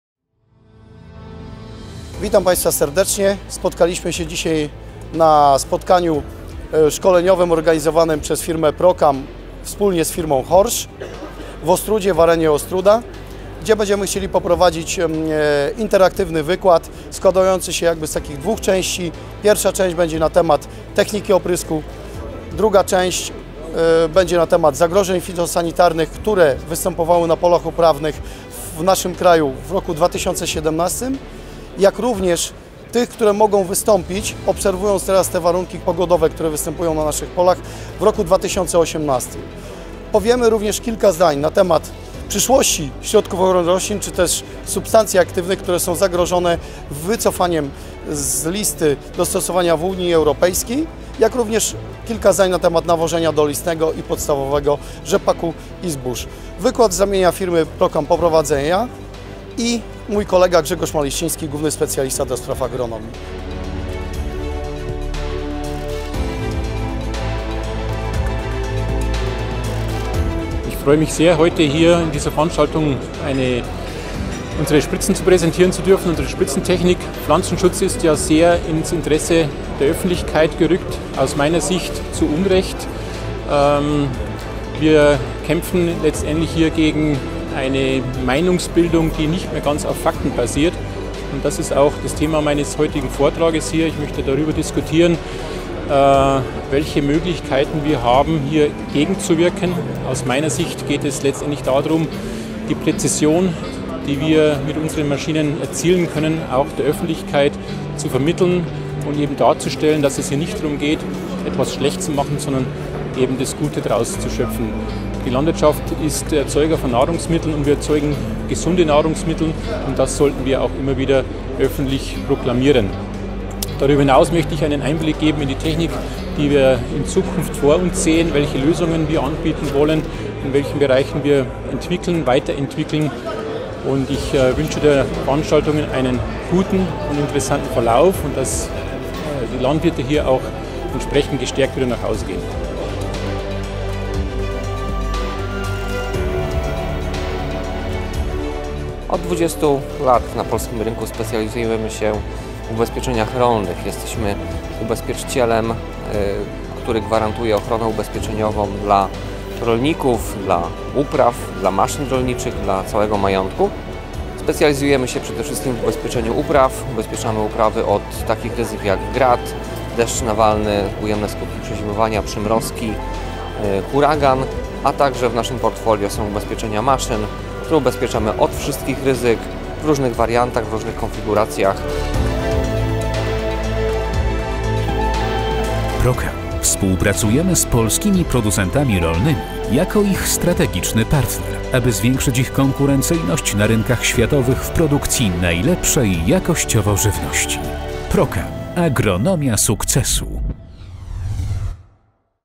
Agroforum w Ostródzie
Prezentujemy relację z AGROFORUM PROCAM i HORSCH jakie odbyło się w Ostrózie gdzie zastanawialiśmy się nad przyszłością rolnictwa.